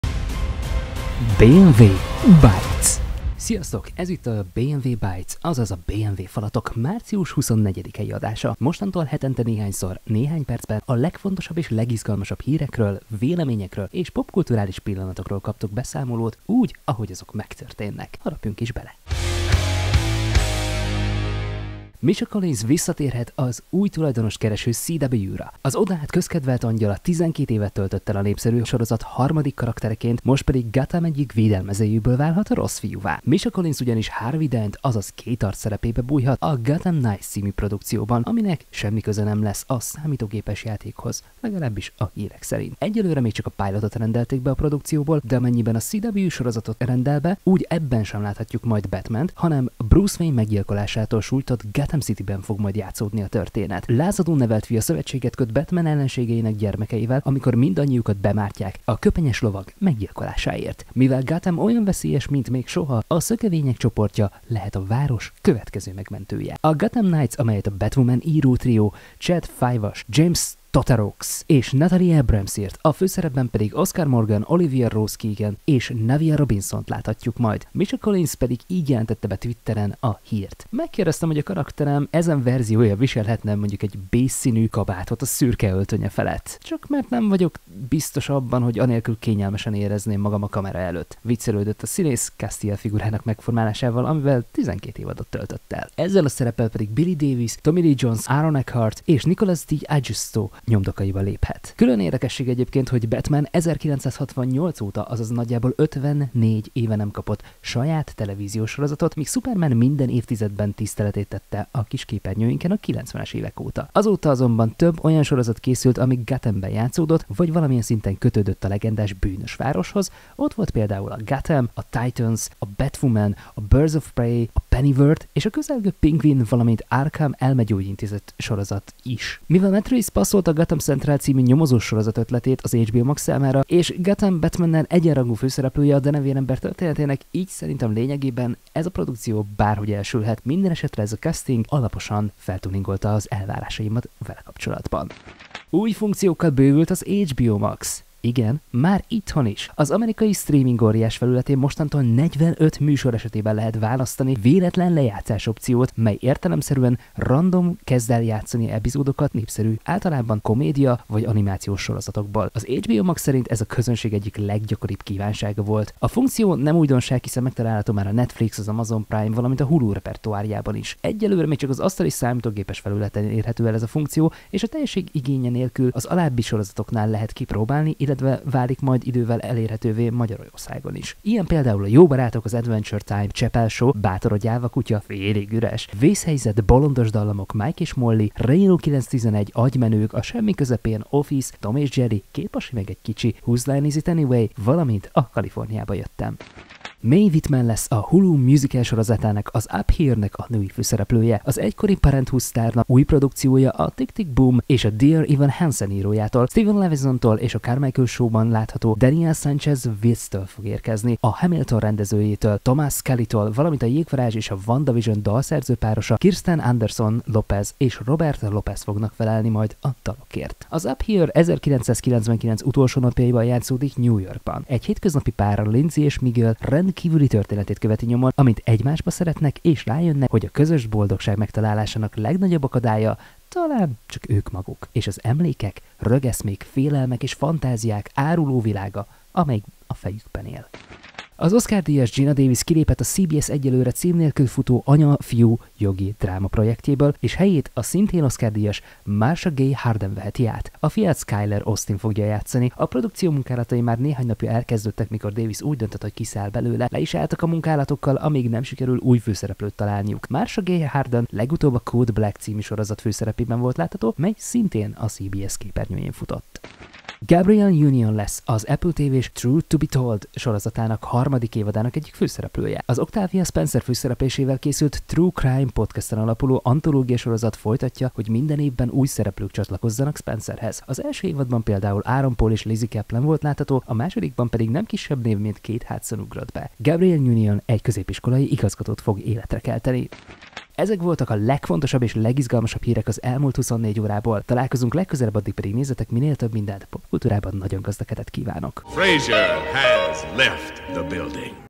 BNV Bites #002.
A BNV Bites mai adásában a Gotham Knights megtalálta Harvey Dentet! Főszereplők lépnek le ú jsorozatból, míg máshol új társ-főszereplőt találnak, valamint új funkcióval bővült az HBO Max.